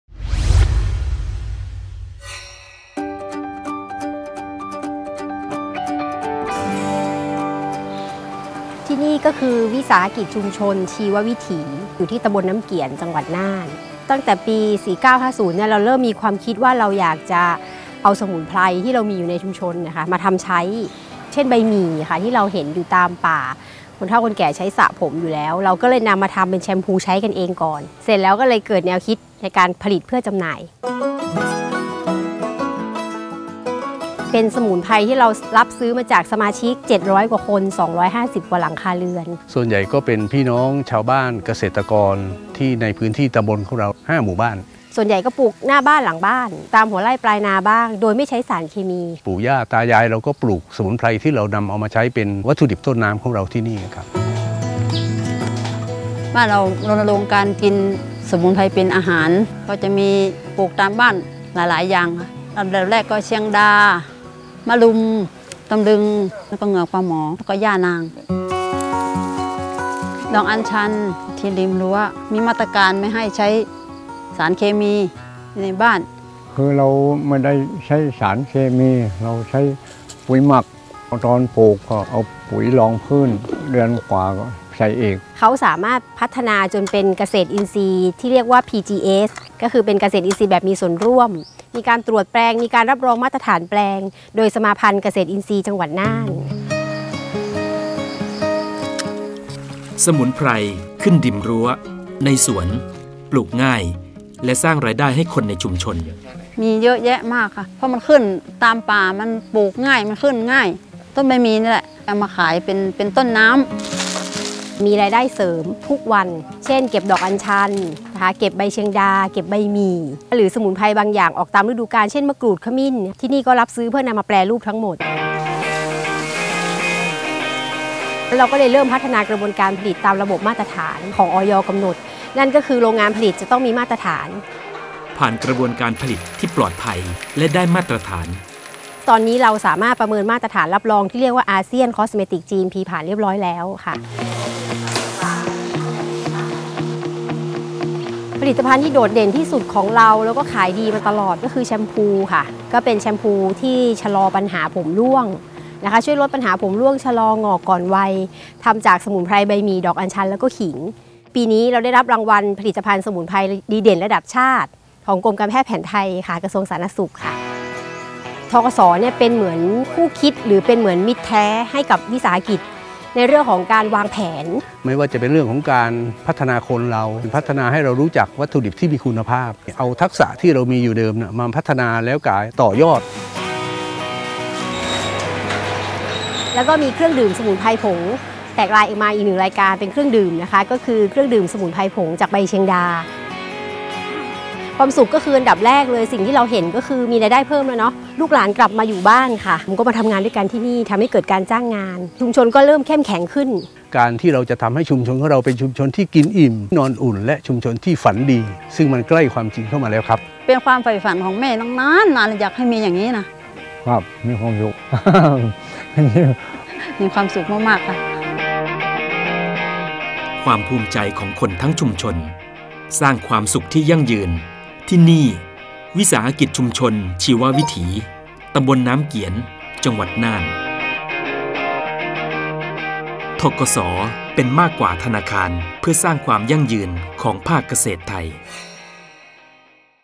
พืชผักใกล้ตัว สู่ผลิตภัณฑ์สมุนไพรชุมชน รางวัลระดับชาติ | บ้านน้ำเกี๋ยน
หยิบสมุนไพรใกล้ตัว มาแปรรูปเป็นของใช้ ใช้ดีเลยทำขาย สร้างรายได้ ชุมชนเข้มแข็ง
ที่บ้านน้ำเกี๋ยน อ.ภูเพียง จ.น่าน ปู่ย่า ตายาย เก็บพืชผักสมุนไพรอินทรีย์ที่มีอยู่ในสวน ขึ้นอยู่ริมรั้วมาขายเป็นรายได้เสริม เกิดการจ้างงาน ลูกหลานไม่ต้องไปรับจ้างไกลบ้าน